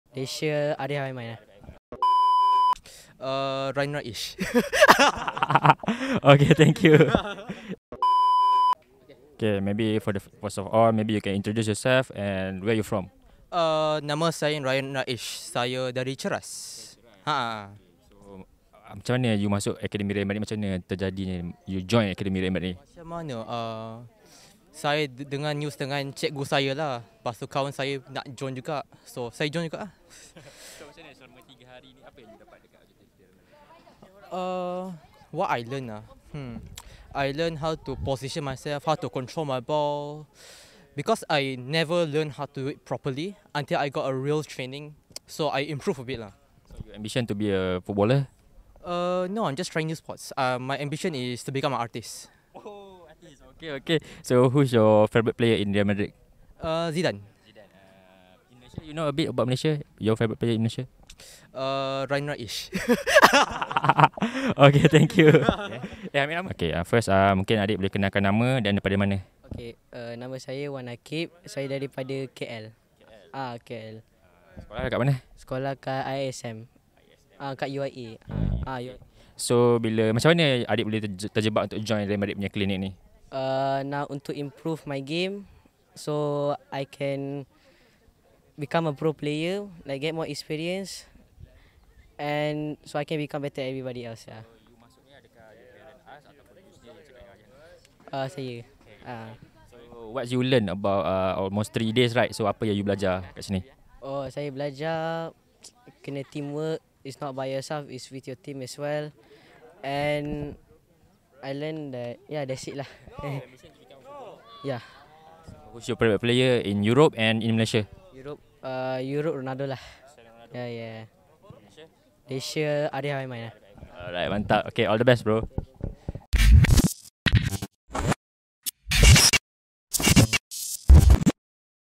"Dekat sini saya belajar teknik dan latihan yang betul sebagai pemain! Idola saya Arif Aiman!"
Temu bual eksklusif peserta Real Madrid Foundation Clinic Malaysia, Raiyan Raesh dan Wan Aqib Fizzly ketika ditemui di International School of Kuala Lumpur (ISKL) untuk sesi clinic bersama jurulatih dari Academy Real Madrid dari 21 - 23 October 2023.